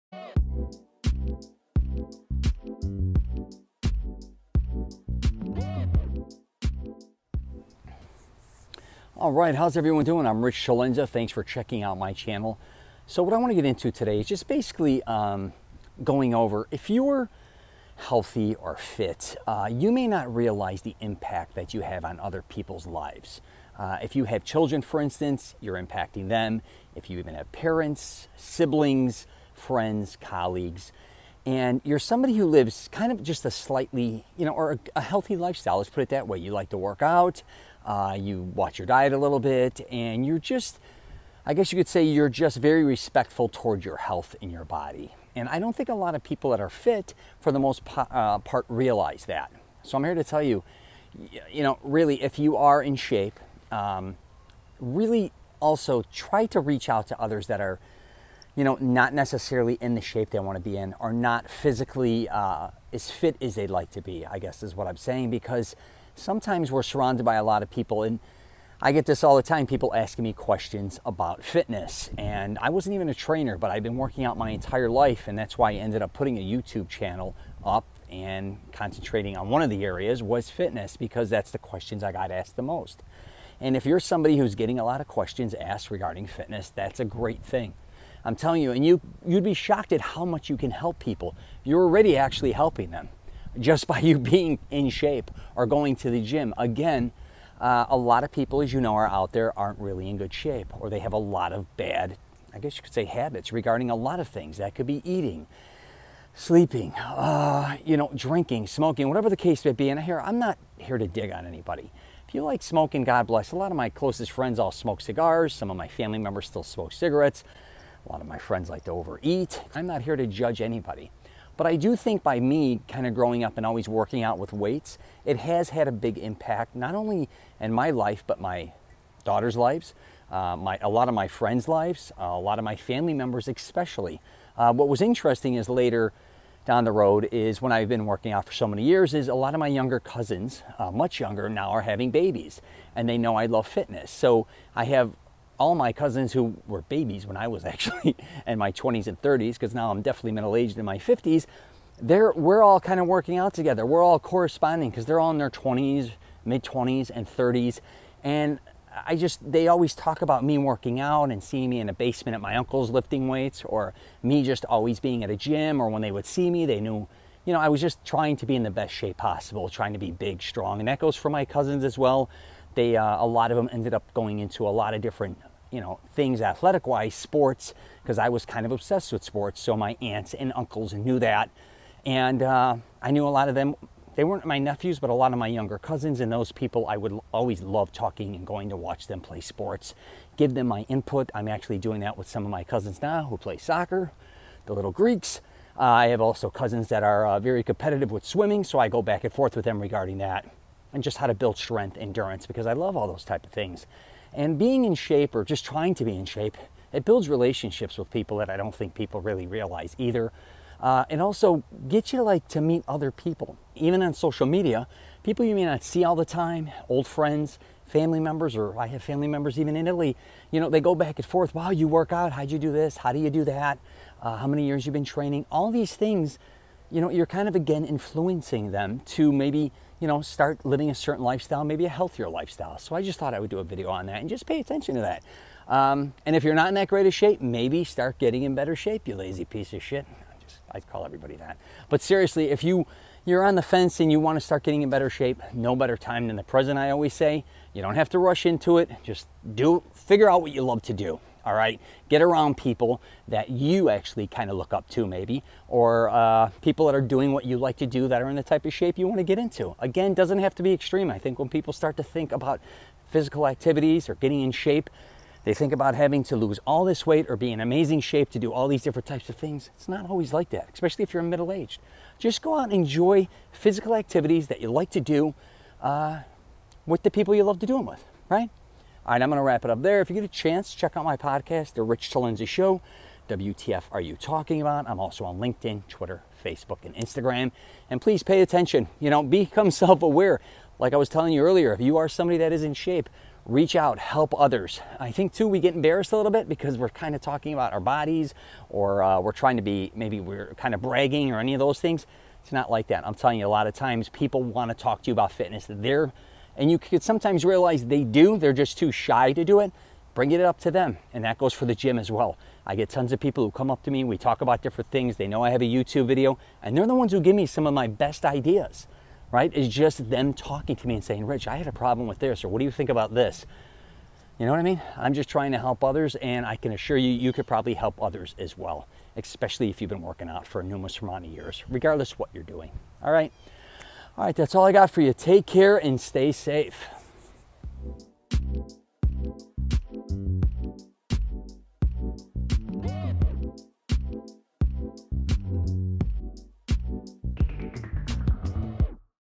Inspire Others By Being Fit!!!
Listen To My Podcast: The Rich Celenza Show "WTF Are You Talking About”

RICH CELENZA discusses how people who work out and live a healthy lifestyle may not realize how much they are influencing others to do the same. That goes for inspiring family members, friends, colleagues, and people you may not even know personally.

People sometimes are not self-aware enough to understand how important them being in shape really is. Not only does it help with their health, but it also leads others to live a healthier lifestyle as well. People who are also consistent and never quit working out also show others that with hard work and dedication you can get exceptional results.